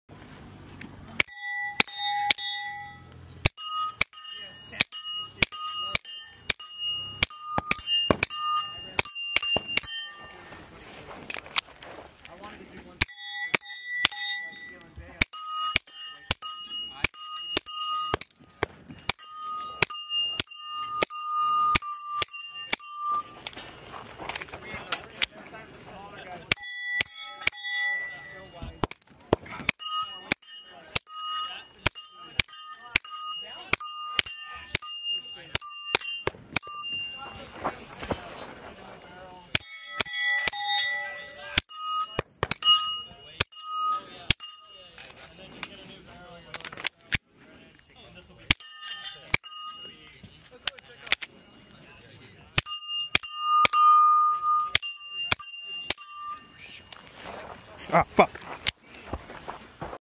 If You Listen Closely, You'll Hear The Moment We Realize There's Another Box
We sent our marketing guy out to get some footage, and he wound up competing in a 2-gun match.